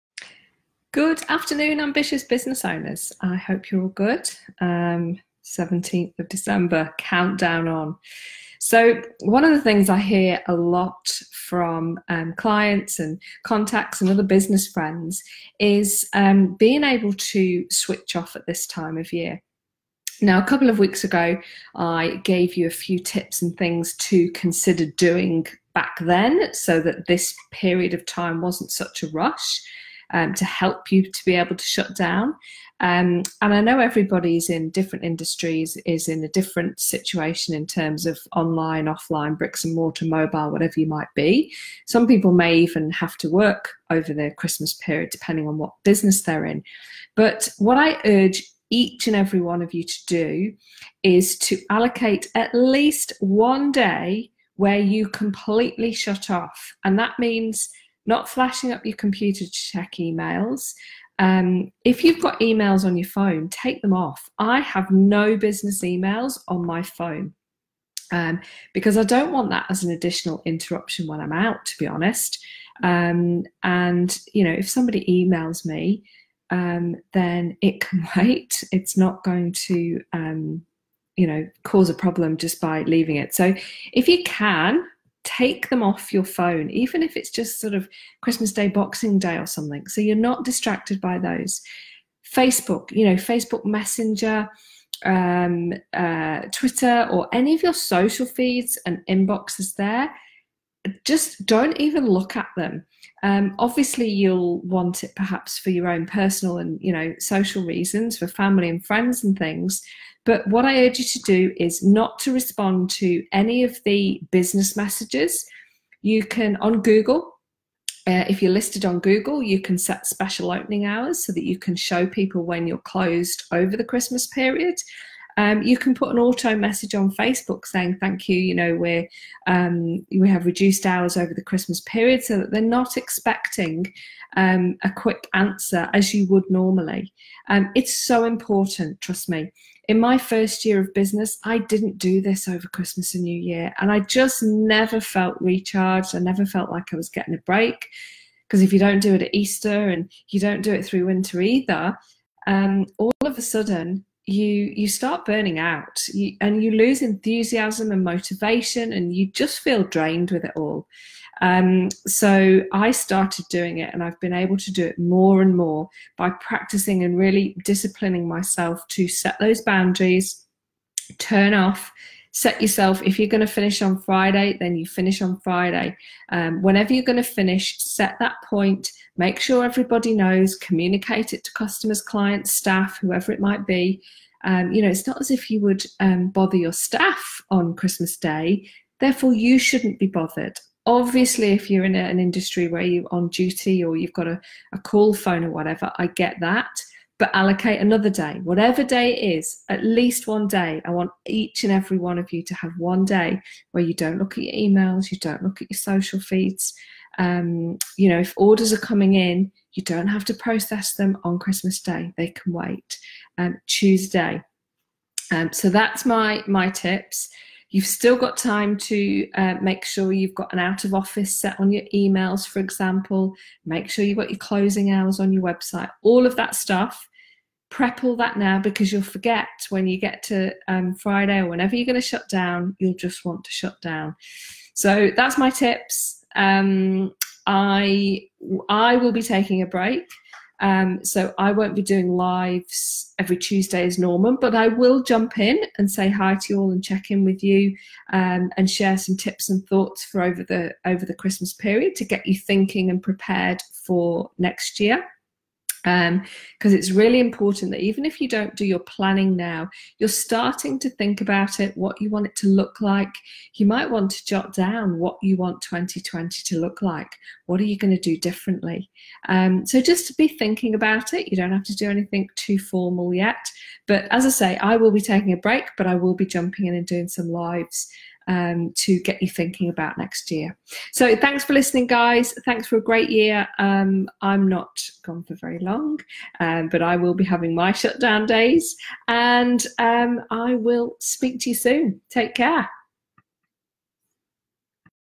Make Time to Take Time
My thoughts and experience on shutting down at this time of year...